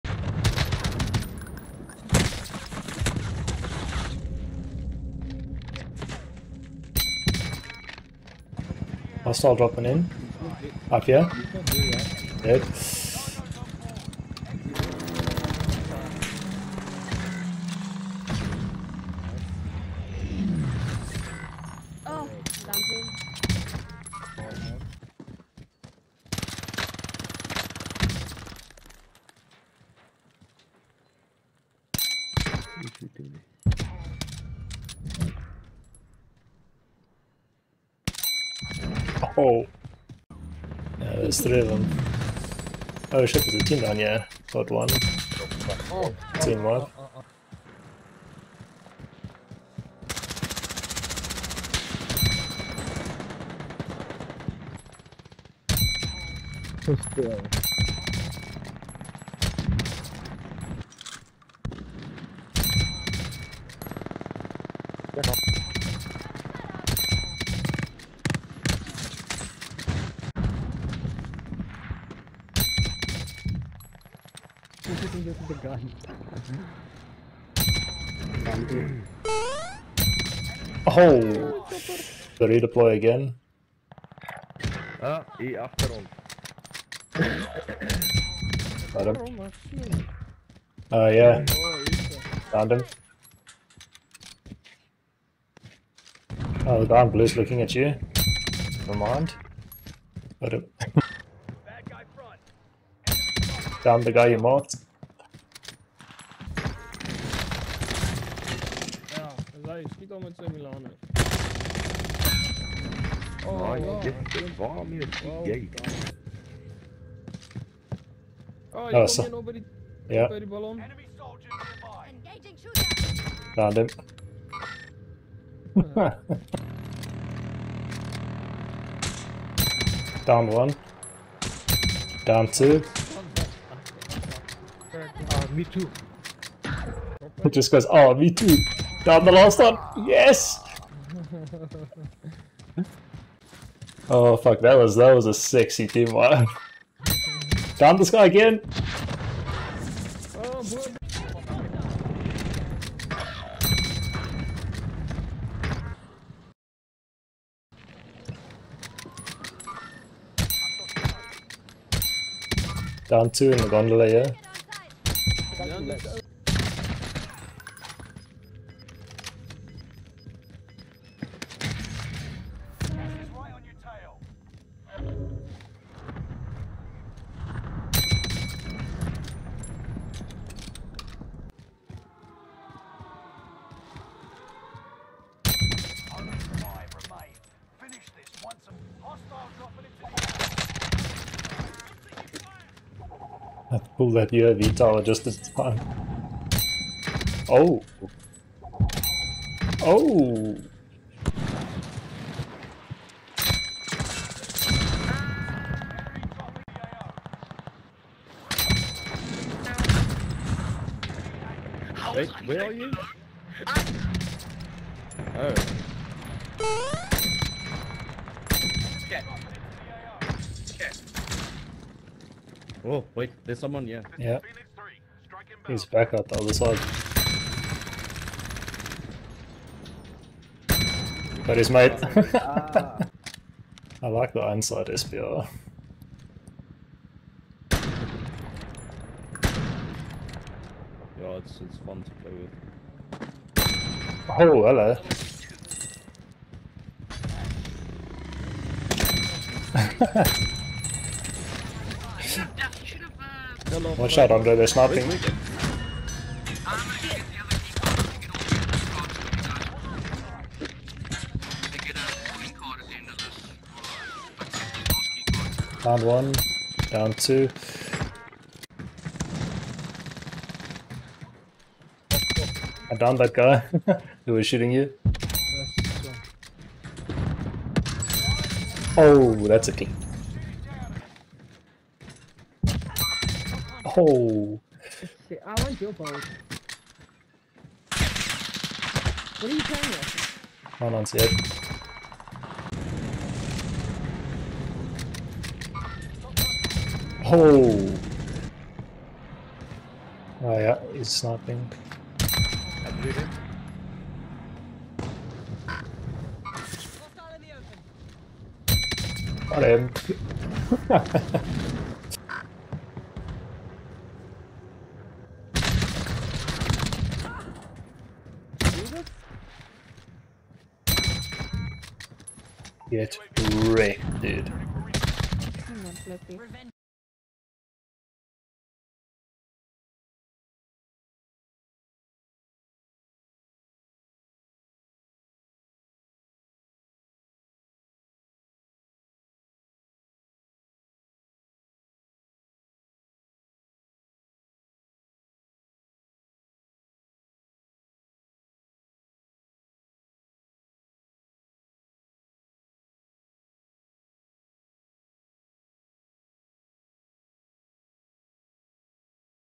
Been hitting some crisp shots lately | Clips #18
I've decided to be a bit more picky with the clips I include in this series in the hopes that they are more entertaining to watch.

No glint Kar98k: monolithic suppressor, 27.6" barrel, tac laser, precision comb stock, scout combat optic with t-pose reticle

No glint SP-R: monolithic suppressor, SP-R 26 barrel, tac laser, norma mag, scout combat optic with t-pose reticle

Iron sight SP-R: 10 round mag, focus perk, tac laser, blitz stock, feather bolt assembly